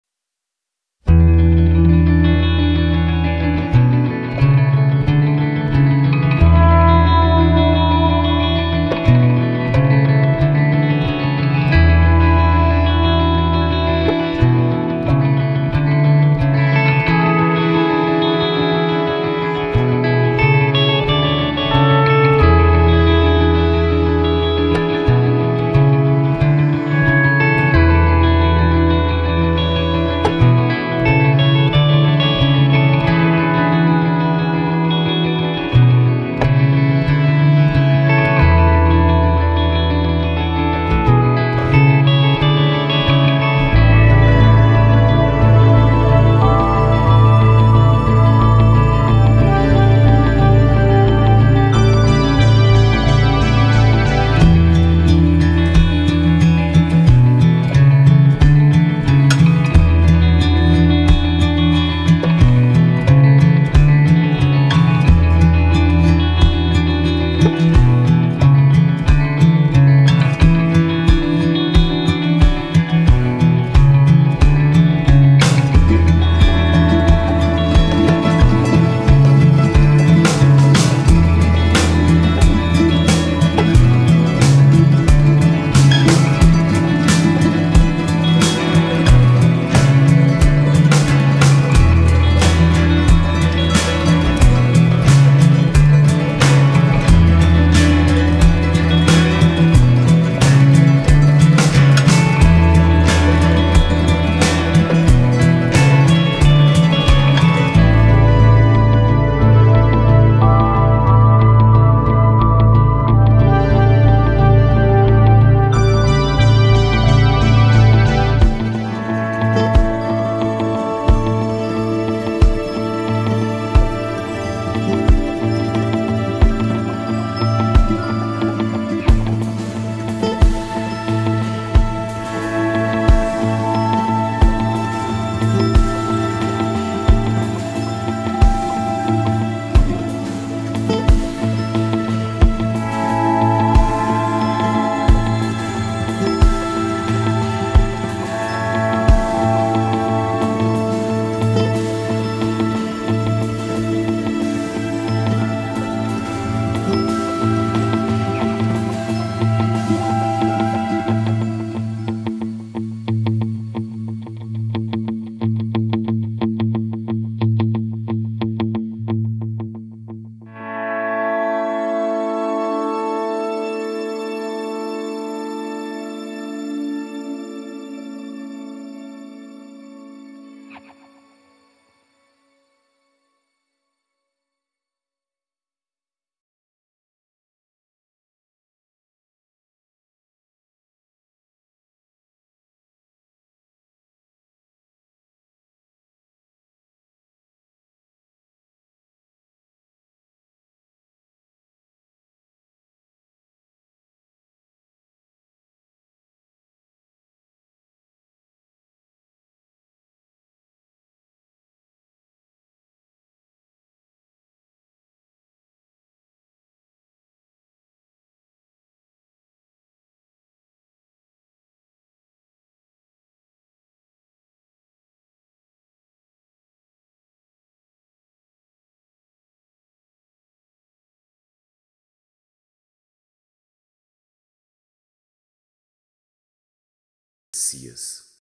YHVH - YAHWEH
YHVH termo atribuído ao nome do Eterno Deus,que abençoou Abraão, Isaque e Jacó. Oremos por Jerusalém!